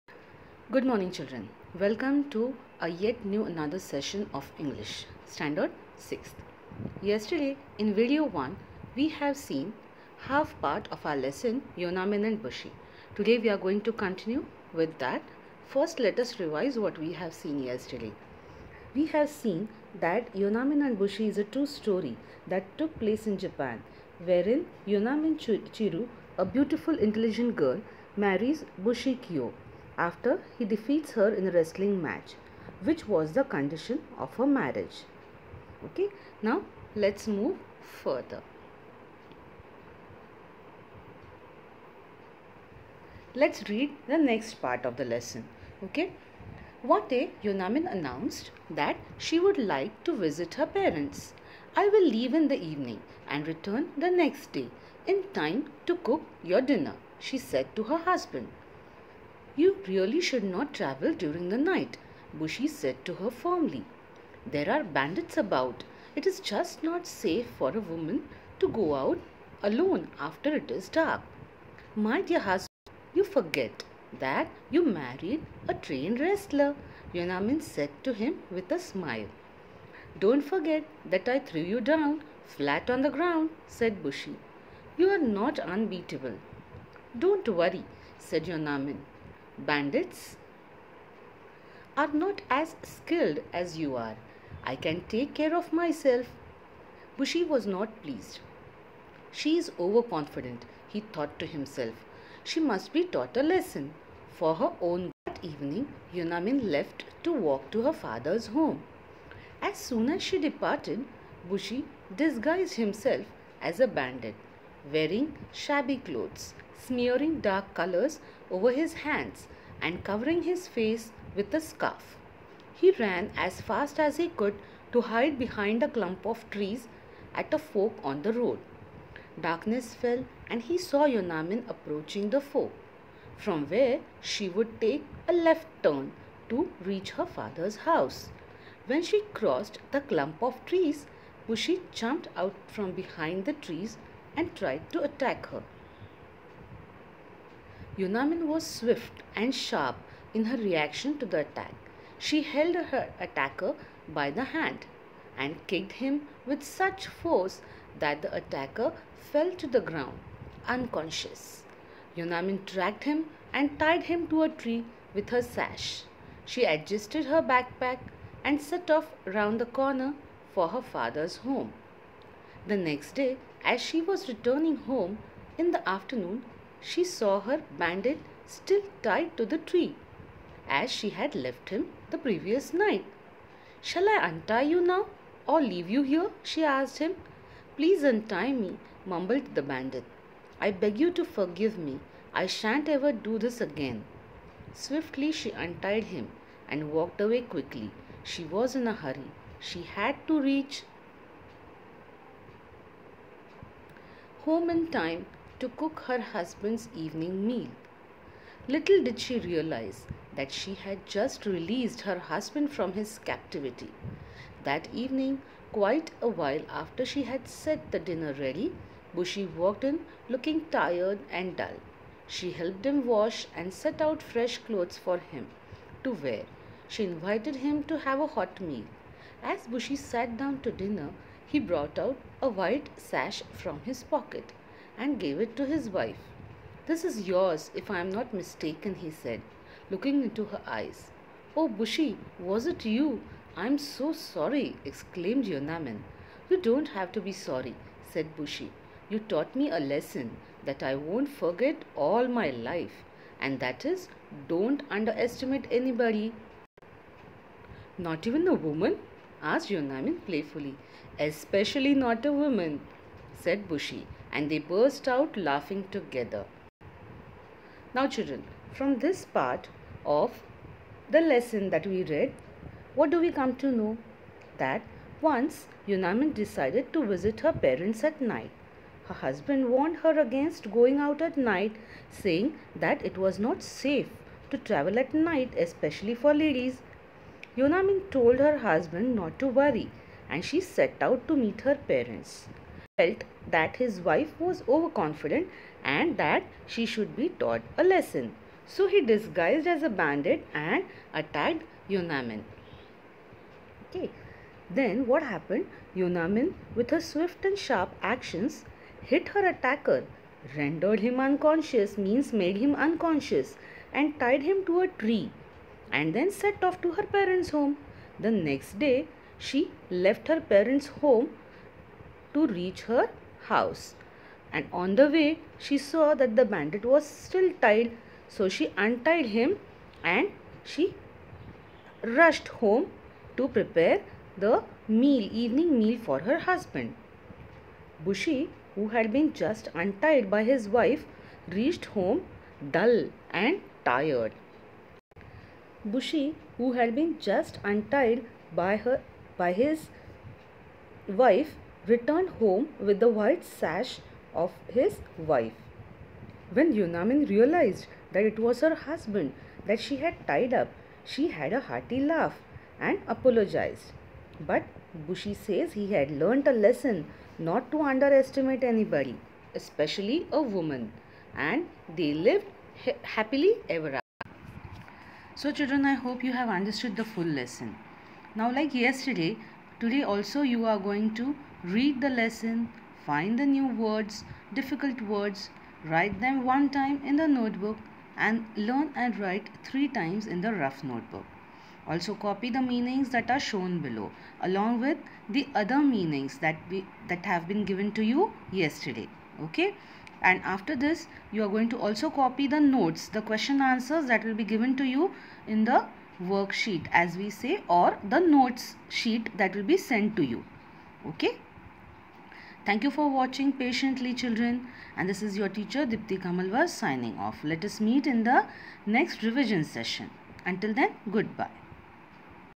Date- 15.10.2020 Std VI Sub:-English I Topic- Yonamine and Bushi - Video 2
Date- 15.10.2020
Std VI
Sub:-English I
Topic- Yonamine and Bushi - Video 2
 Description-This video explains the true  Japanese story of Yonamine Chiru and Bushi Kiyo .
 After the explanation do the work given in the video.